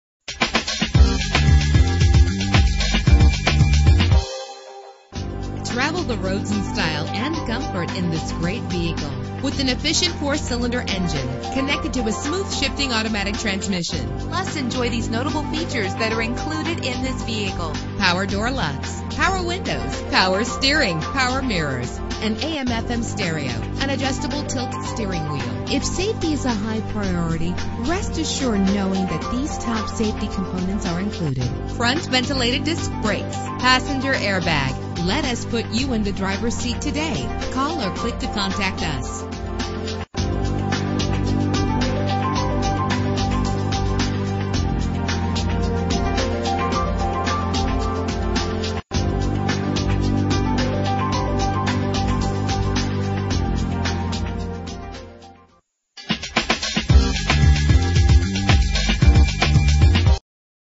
1998 Honda Civic - Albany OR
Year: 1998
Make: Honda
Model: Civic
Trim: HX
Engine: 1.6 liter inline 4 cylinder 16 valve
Transmission: Automatic CVT
Color: Flamenco Black Pearl
Mileage: 172146
Address:
5040 Pacific Blvd SW